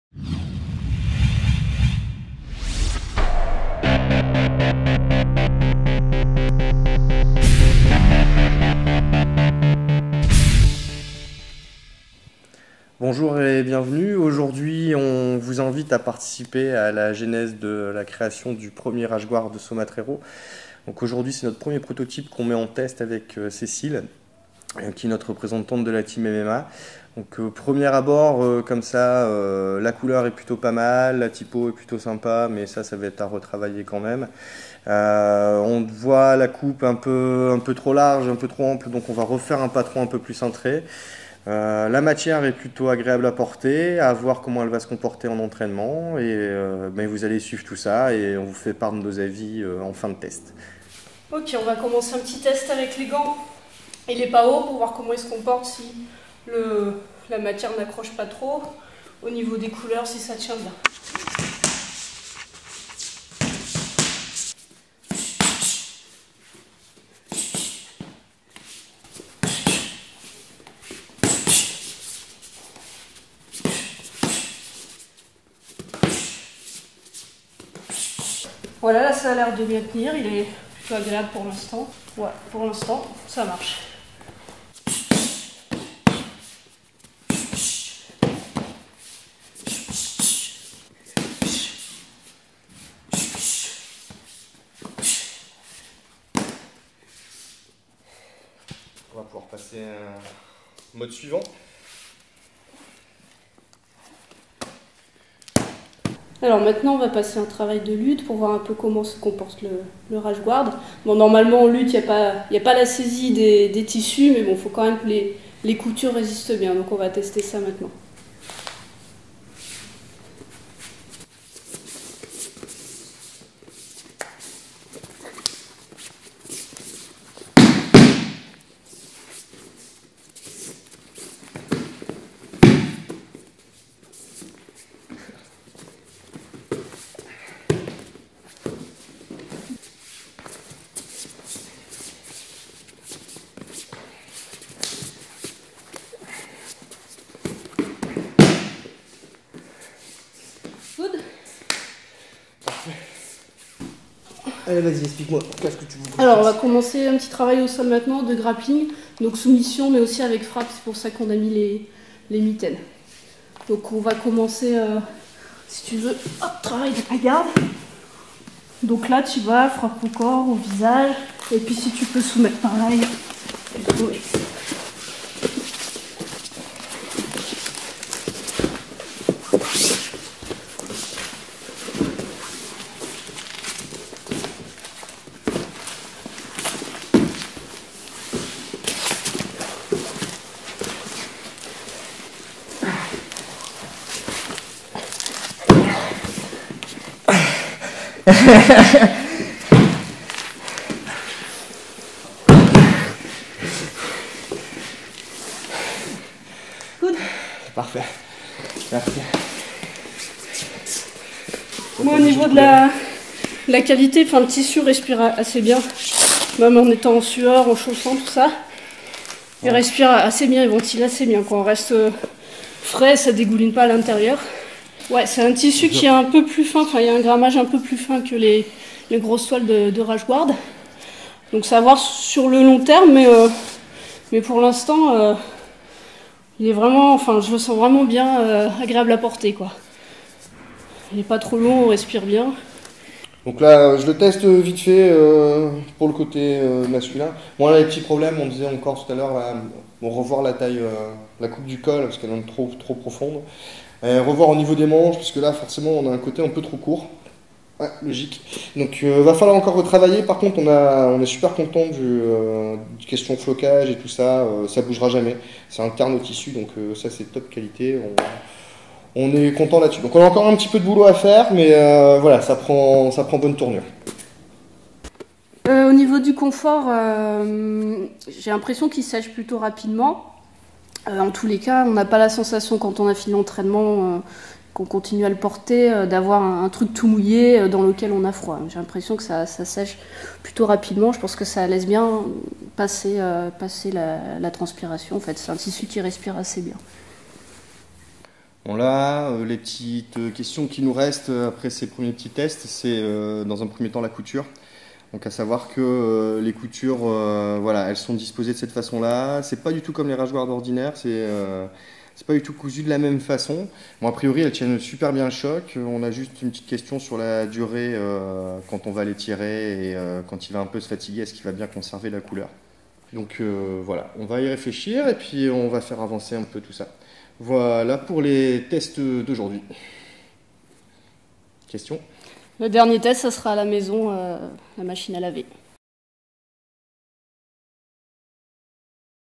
MMA - Test en action du 1er Rashguard Só Matreiro
Les vidéos crash tests de nos différents prototypes et produits commencent!
On démarre donc avec notre premier prototype de rashguard pour la pratique de l'urban bujitsu et du MMA.

Objectif, vous faire partager l'envers du décor et vous permettre de participer aux processus de test et de création. Alors n'hésitez pas à nous faire part de vos commentaires et idées!

Julien, généralement plus habitué aux arts martiaux traditionnels, se lance à corps perdu dans sa première séance de MMA en compagnie de notre spécialiste Cécile Fontaine.